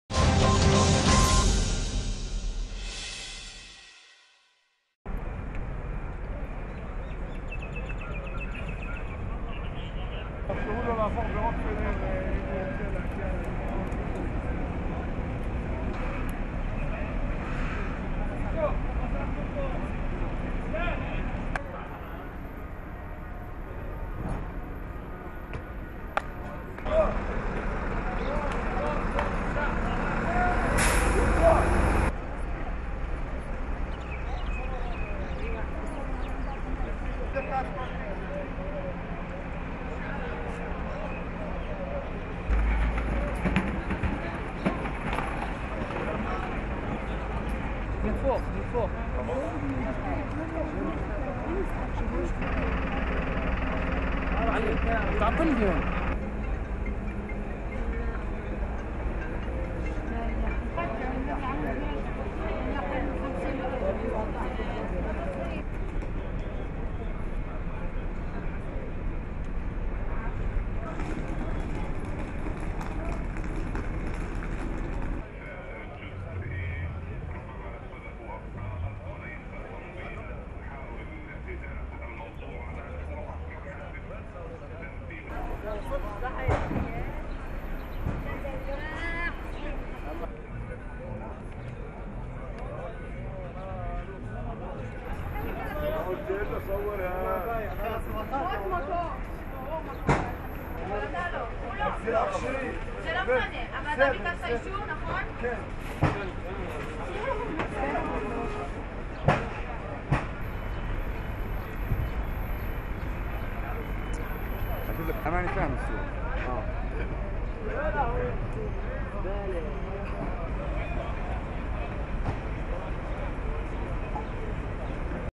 Israel bars Palestinians under 50 from entering Al Aqsa
JERUSALEM - JULY 31:  Israel bars Palestinians under 50 from entering Al-Aqsa Mosque and Palestinians protest Israeli restrictions in Jerusalem on July 31, 2015. ...